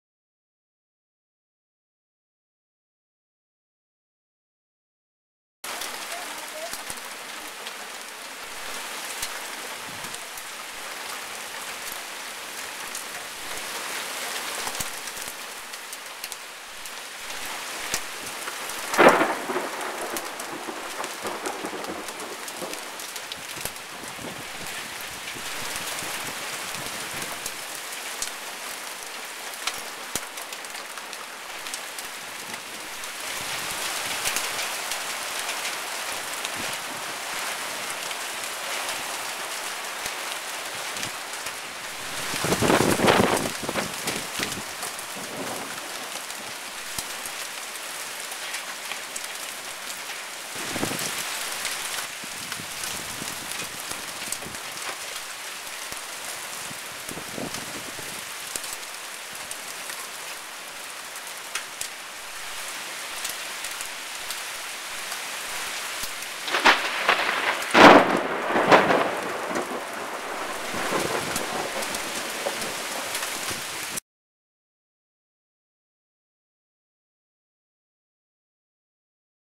Summer Storm in Talking Rock
This short storm was very powerful. It knocked out power out for about 9 hours.